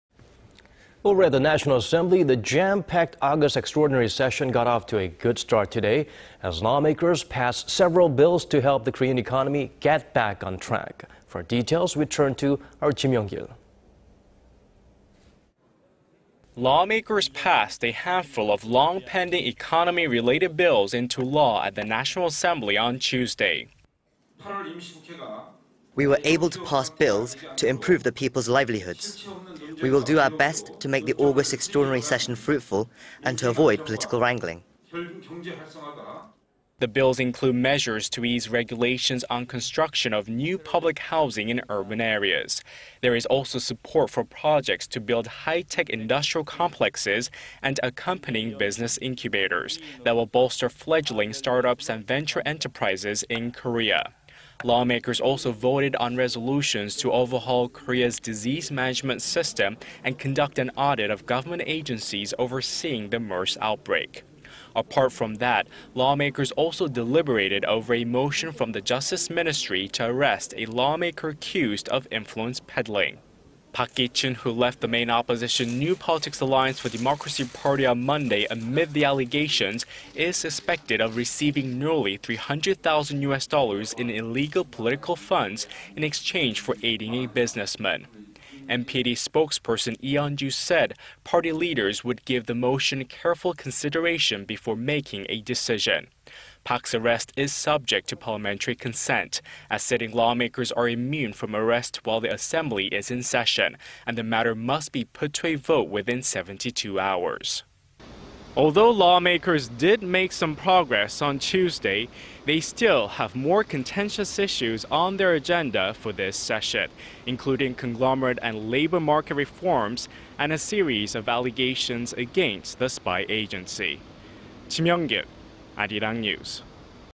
Assembly passes economy－related bills 8월 임시국회 첫 본회의 계류법안 및 결의안 등 처리
Over at the National Assembly，... the jam－packed August extraordinary session got off to a good start today... as lawmakers passed several bills to help the Korean economy get back on track.
For details we turn to Ji Myung－kil.
Lawmakers passed a handful of long－pending economy－related bills into law at the National Assembly on Tuesday.

″We were able to pass bills to improve the people′s livelihoods. We will do our best to make the August extraordinary session fruitful and to avoid political wrangling.″
The bills include measures to ease regulations on construction of new public housing in urban areas.
There is also support for projects to build high－tech industrial complexes... and accompanying business incubators... that will bolster fledgling startups and venture enterprises in Korea.
Lawmakers also voted on resolutions to overhaul Korea′s disease management system and conduct an audit of government agencies overseeing the MERS outbreak.
Apart from that， lawmakers also deliberated over a motion from the justice ministry to arrest a lawmaker accused of influence peddling.
Park Ki－choon ， who left the main opposition New Politics Alliance for Democracy party on Monday amid the allegations，... is suspected of receiving nearly 300－thousand U.S. dollars in illegal political funds in exchange for aiding a businessman.
NPAD spokesperson Lee Un－ju said party leaders would give the motion careful consideration before making a decision.
Park′s arrest is subject to parliamentary consent， as sitting lawmakers are immune from arrest while the assembly is in session， and the matter must be put to a vote within 72 hours.

″Although lawmakers did make some progress on Tuesday， they still have more contentious issues on their agenda for this session，... including conglomerate and labor market reforms and a series of allegations against the spy agency.